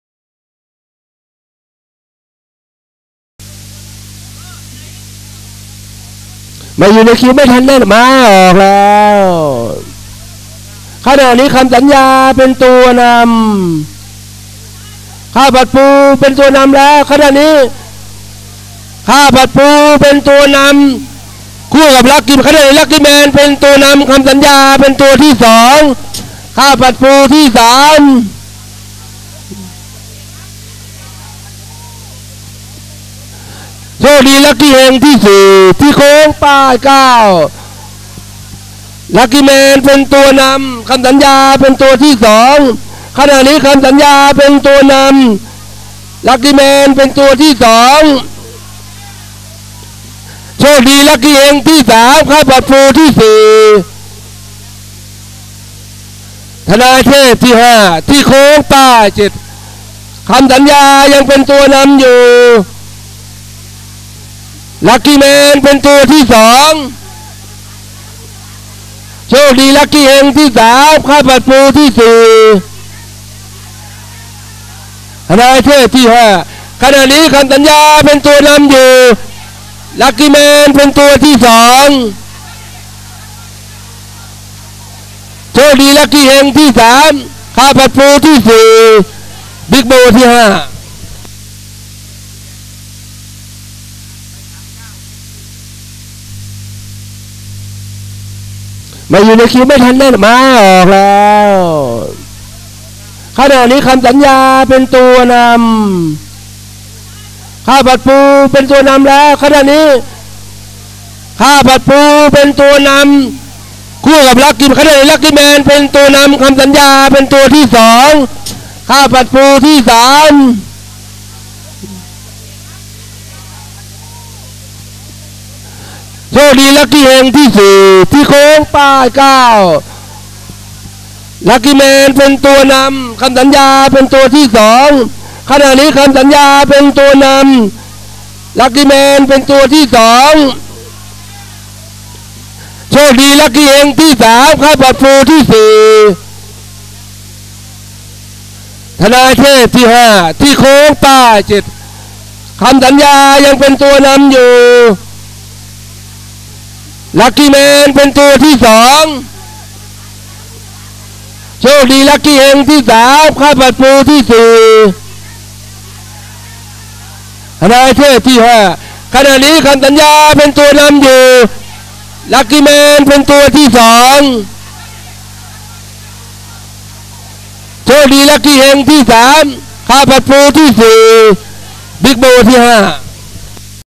[ม้าแข่งสนามโคราช 11 ก.พ. 2555] ม้าเทศชั้น 3ข คำสัญญา+ลักกี้แมน+โชคดีลักี้เฮง
ผลการแข่งม้ากีฬาทหาร จังหวัดนครราชสีมา
สนามม้าโคราช ประจำวันที่  11 กุมภาพันธ์ 2555